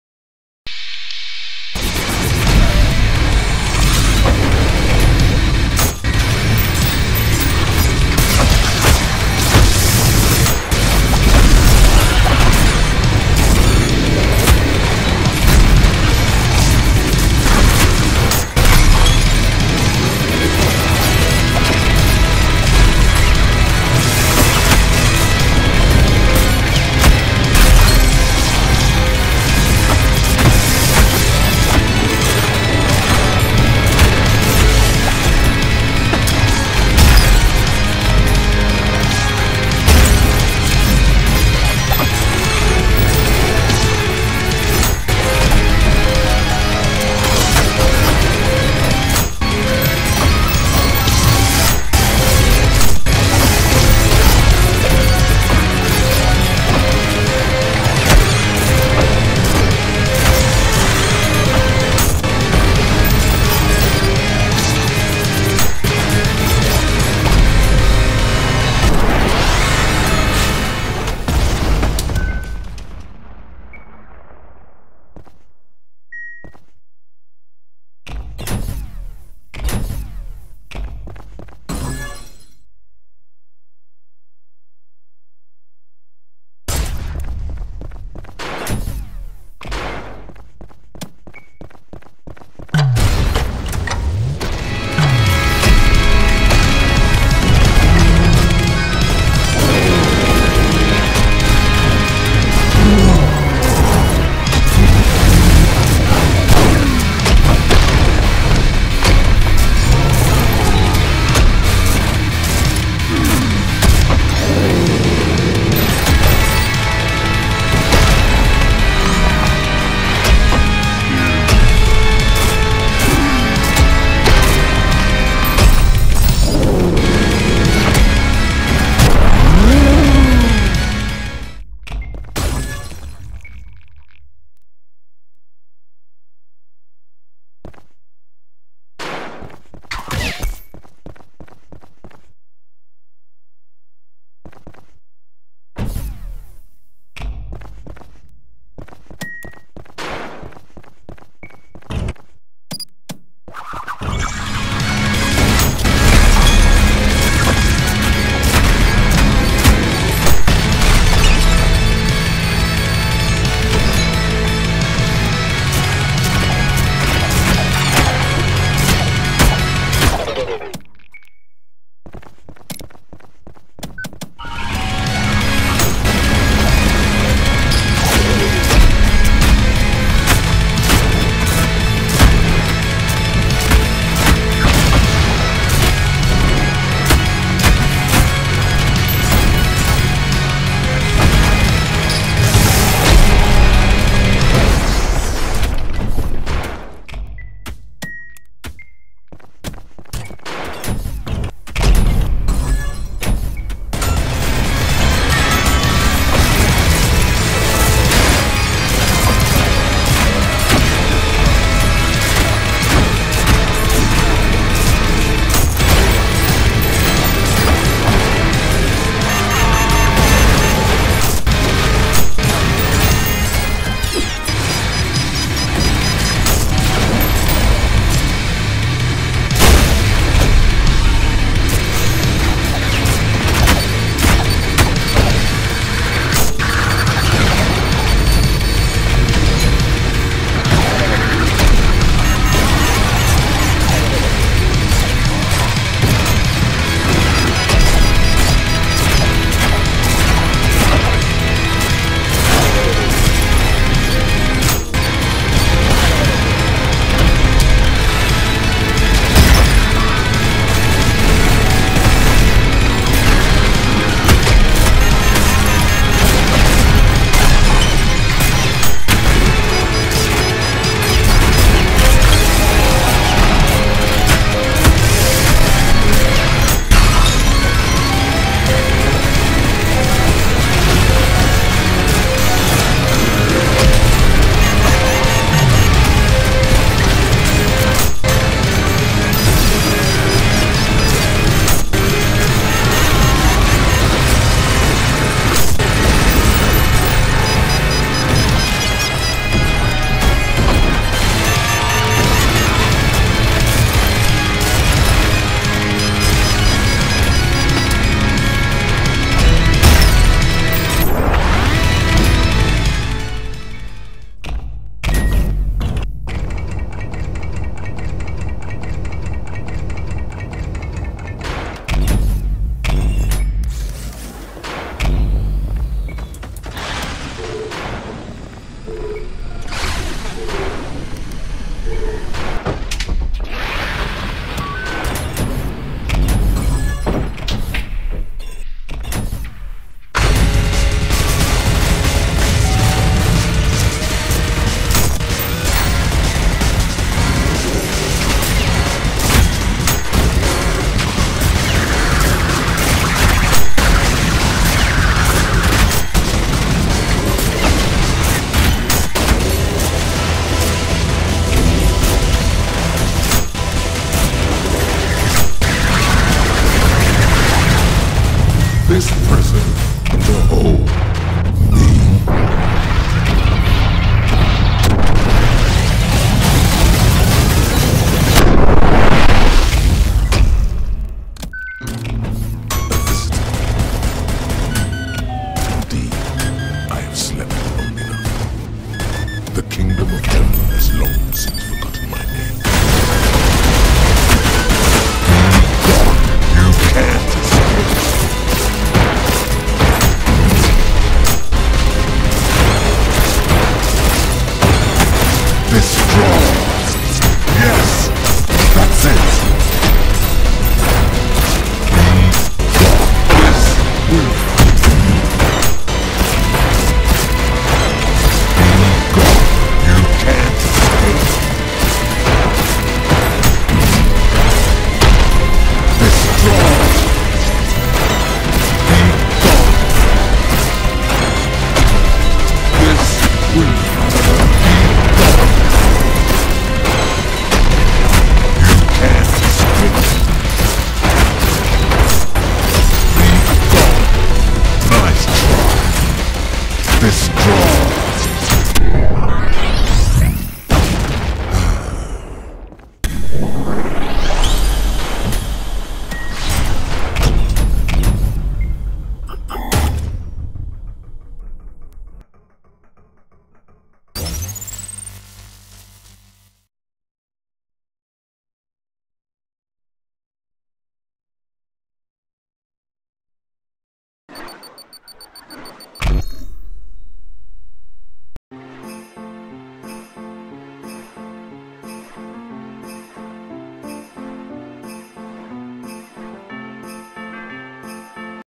P-2 without music between arenas is eerie
I sensed Something Wicked earlier during this run. Coulda sworn that one of the mindflayers ended up killing him during this particular arena the recording started too.

I somehow ended up getting a P-Rank here too. Lost some time because I was spooked and checking behind me every time I cleared an arena.